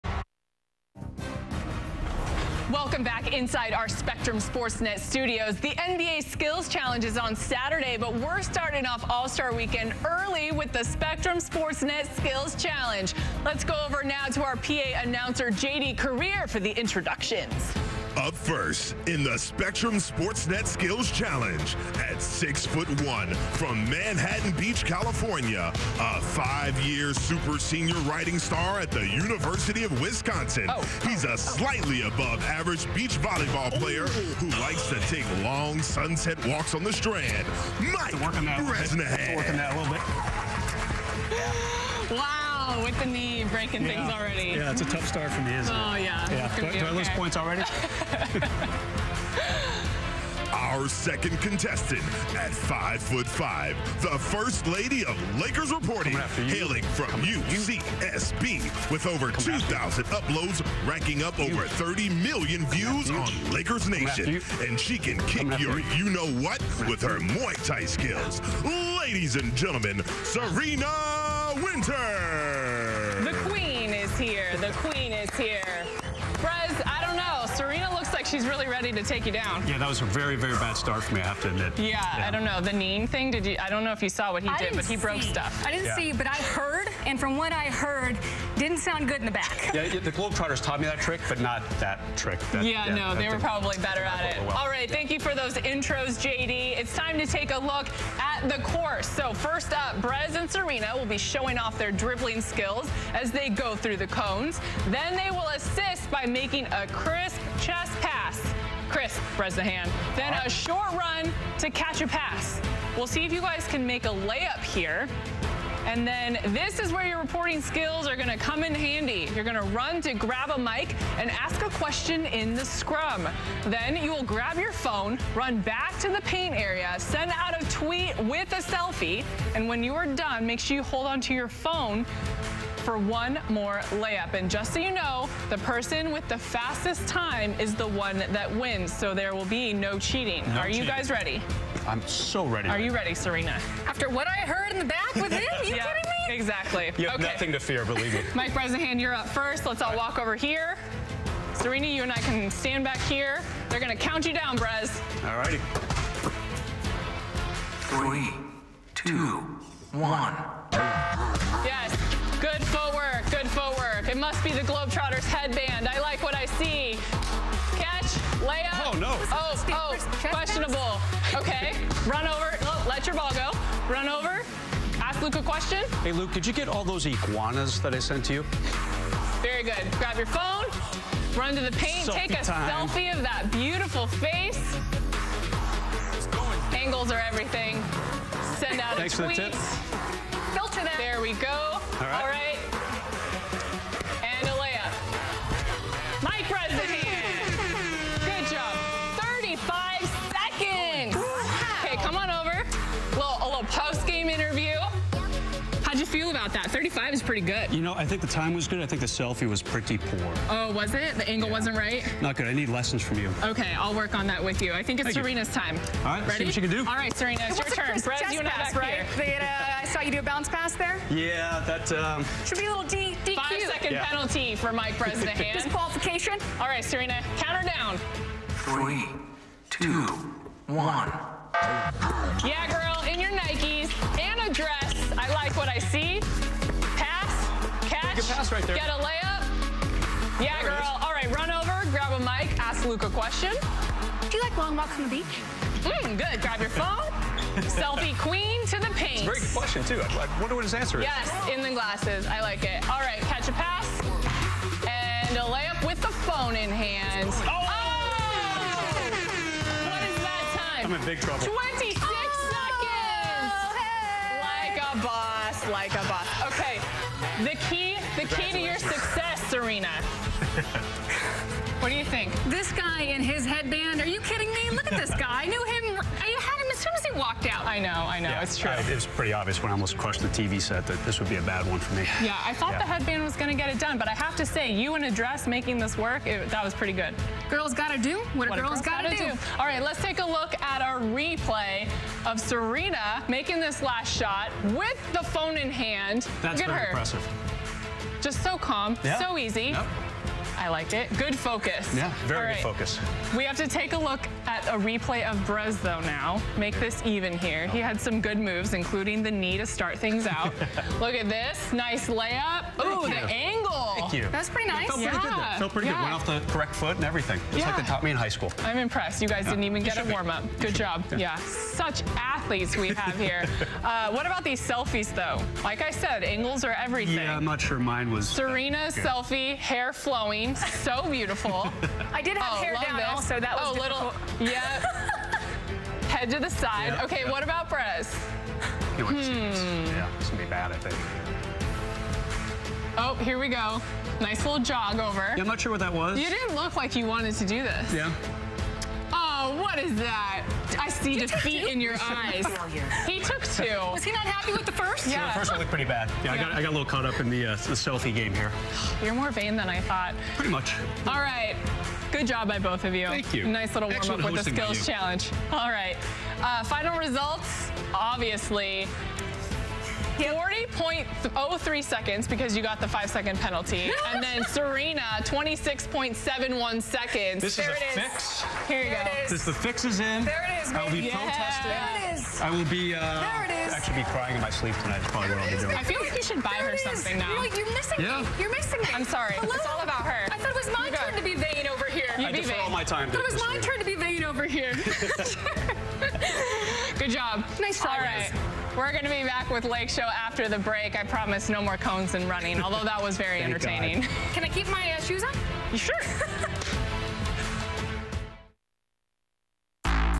Serena Winters Vs. Mike Bresnahan: Lakeshow Skills Challenge
Serena Winters goes head to head with Mike Bresnahan on Spectrum SportsNet!